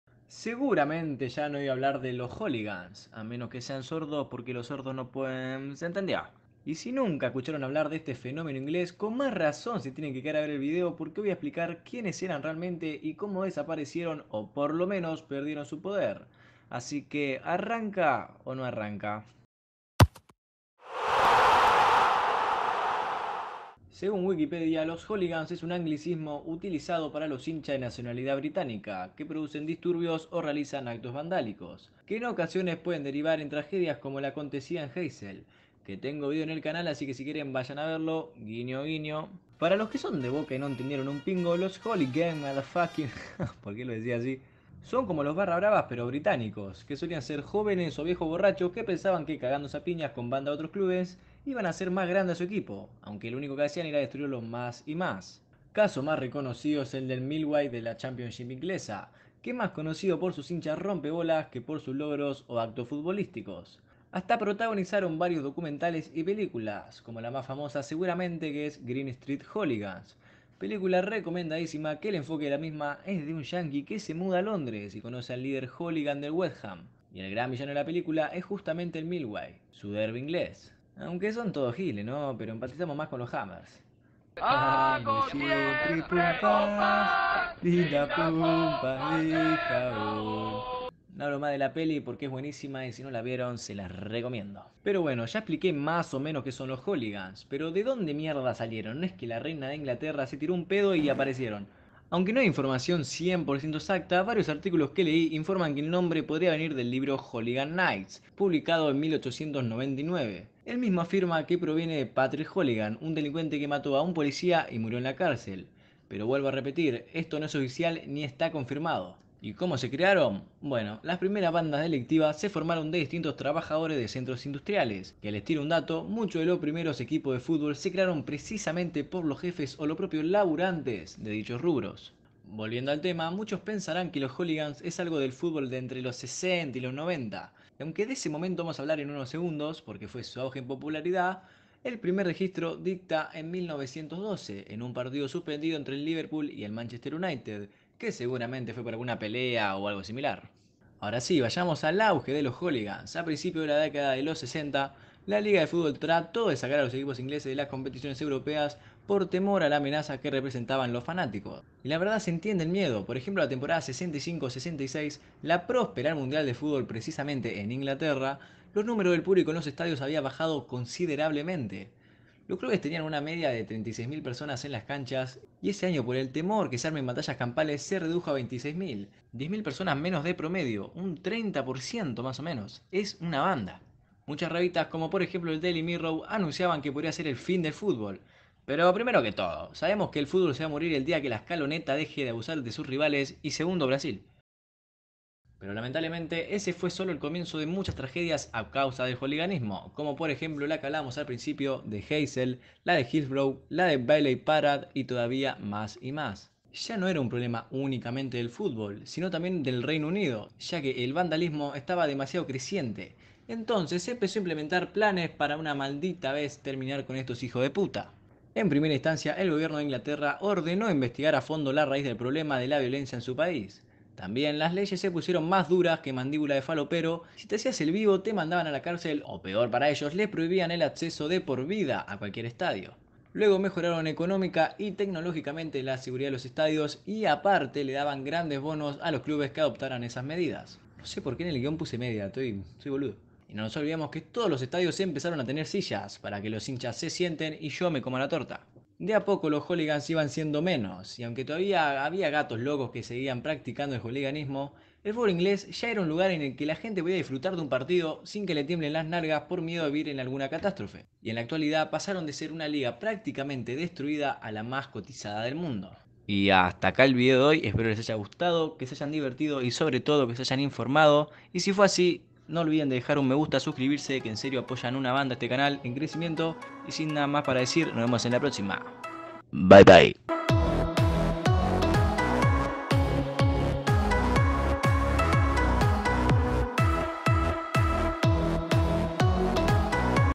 HOOLIGANS: ¿Quiénes eran y como desaparecieron?
El "Hooliganismo" fue una ola de violencia en el futbol en la década de los 60´s hasta maso menos los 2000´s. Pero, ¿sabían que el primer registro fue en 1912? o ¿de dónde salieron? y ¿cómo perdieron su poder? Vean el video completo para sacarse todas dudas posibles ♡